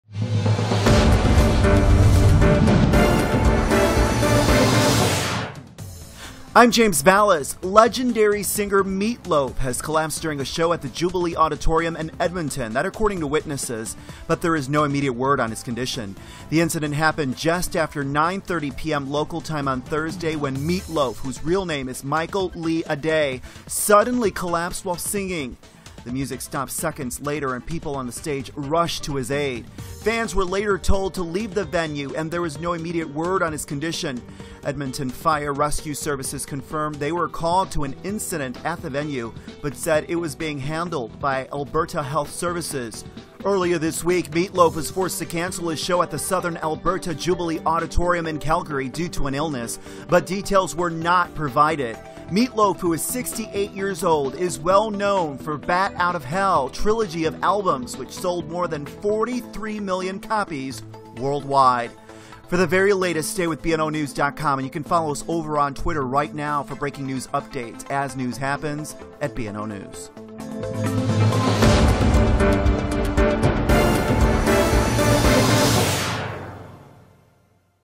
Singer Meat Loaf collapses during show in Edmonton
Legendary singer Meat Loaf has collapsed during a show at the Jubilee Auditorium in Edmonton, witnesses say, but there is no immediate word on his condition. James Valles reporting (BNO News)